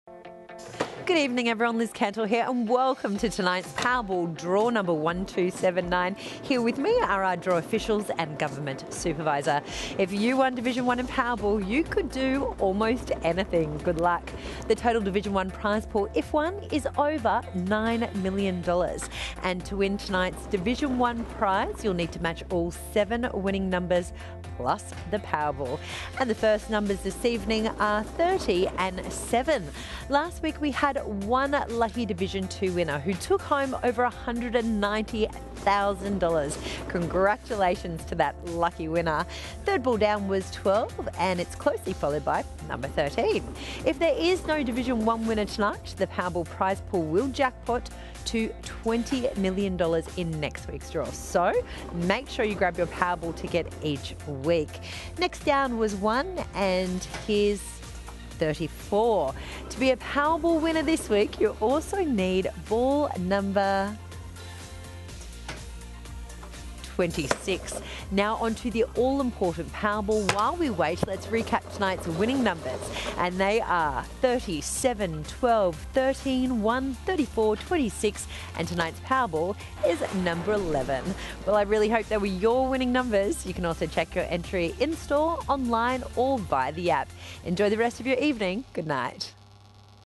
Powerball Results Draw 1279 | Thursday, 19 November 2020 | The Lott
Official Powerball Draw Results from The Lott - Thursday, 19 November 2020 Draw No. 1279.  Have fun and play responsibly.  Gamble Responsibly. Gamblers Help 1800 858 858